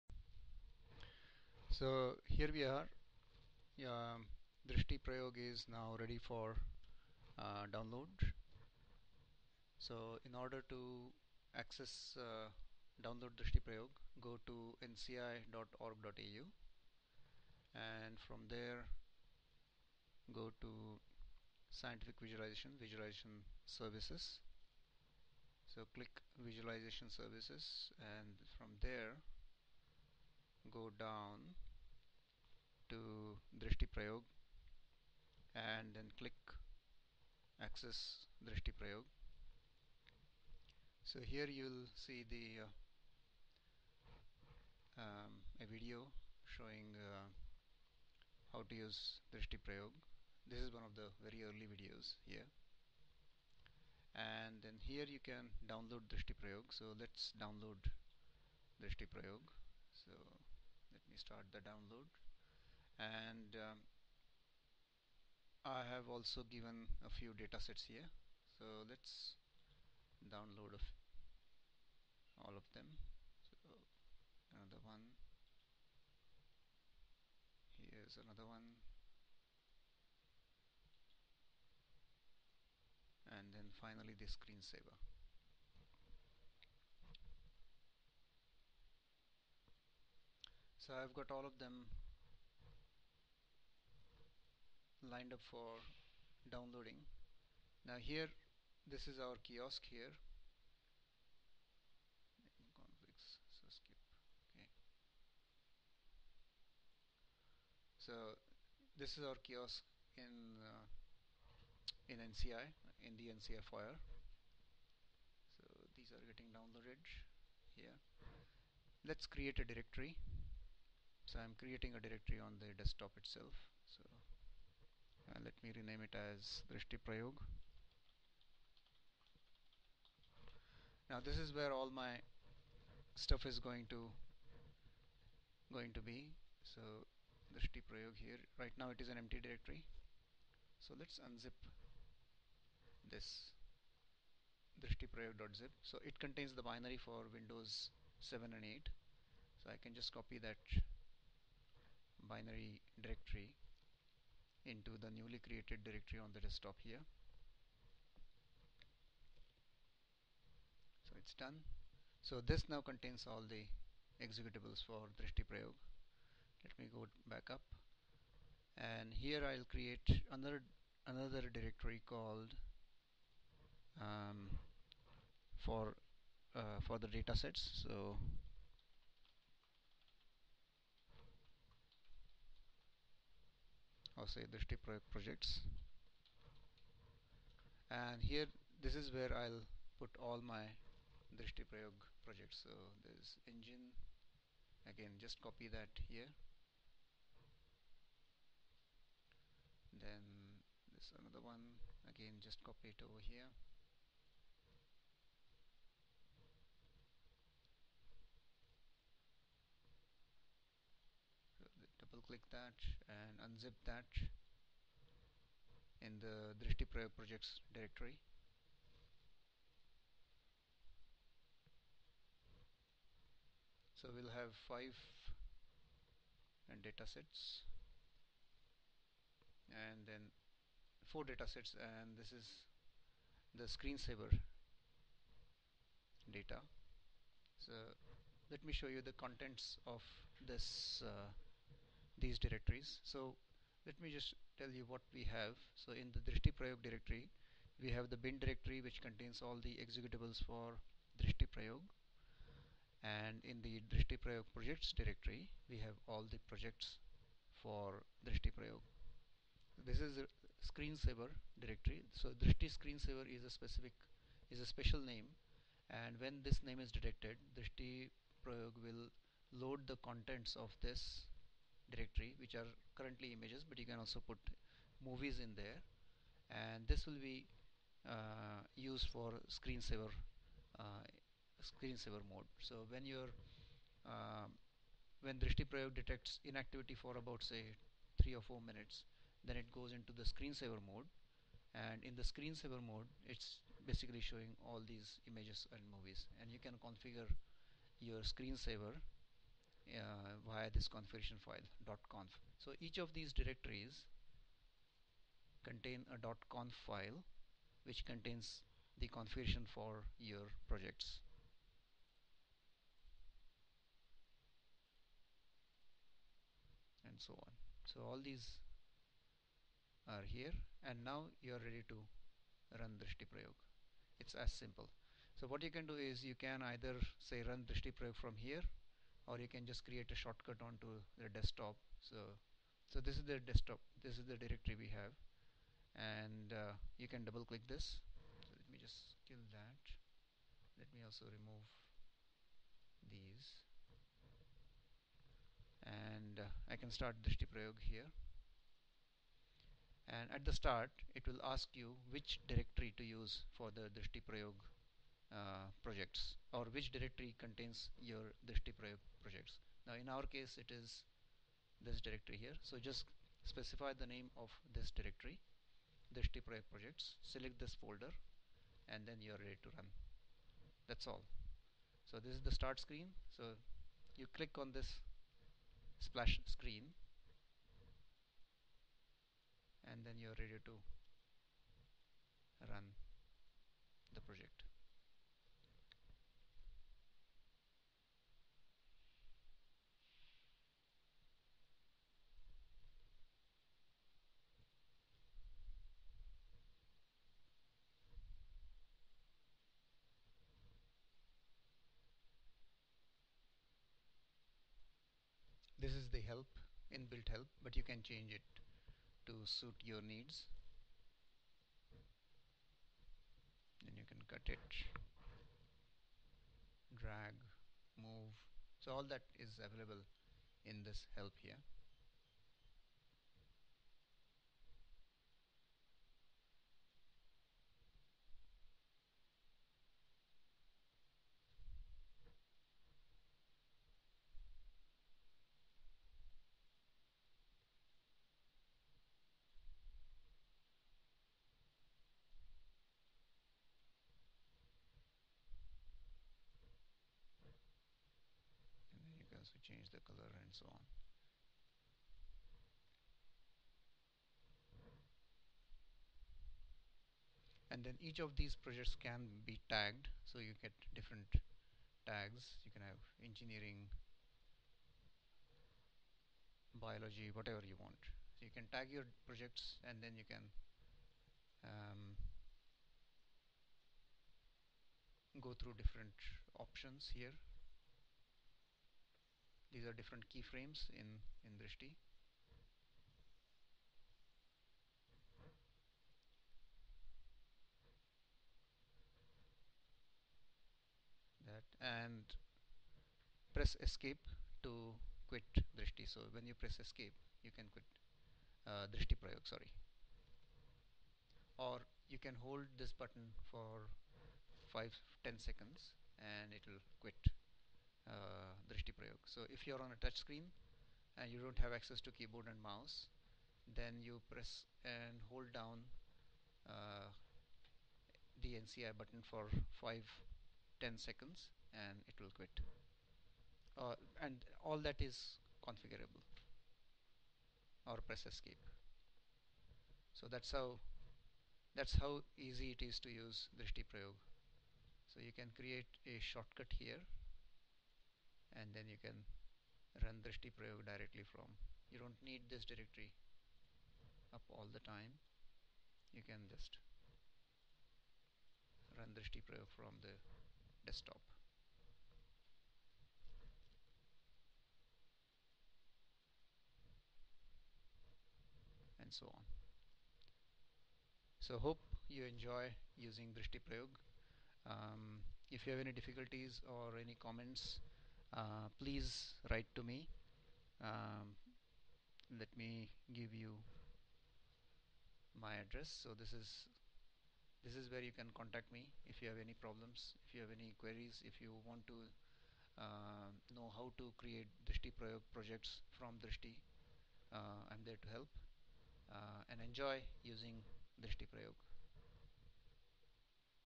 Drishti Prayog - download and run
This video describes how to download and run Drishti Prayog and its projects from nci.org.au

Download DrishtiPrayog.zip
Download all Drishti Prayog projects.

Extract the contents of DrishtiPrayog.zip into a directory.  This directory now contains another directory called "bin" which contains Drishti Prayog executables, required media and library files and configuration file.

Create another directory to hold Drishti Prayog project directories.
Extract projects into this directory.  This projects directory now contains a directory for every project.

Run drishtiprayog.exe and point to the toplevel projects directory when asked for.  You should now be up and running Drishti Prayog.